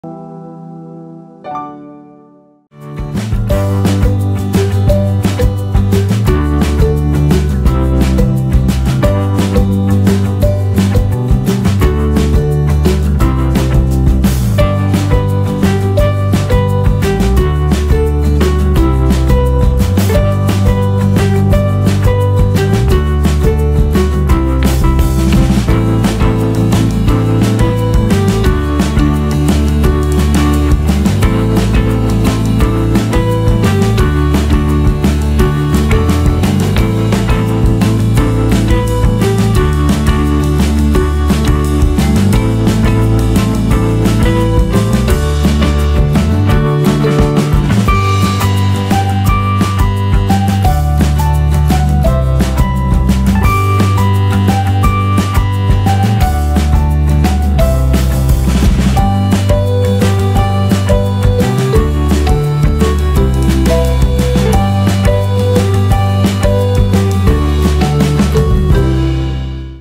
Cat amusing plays (Funny Cute Cat Videos)
Funny cat Sonia amusing plays.
Забавная кошка Соня весело играет.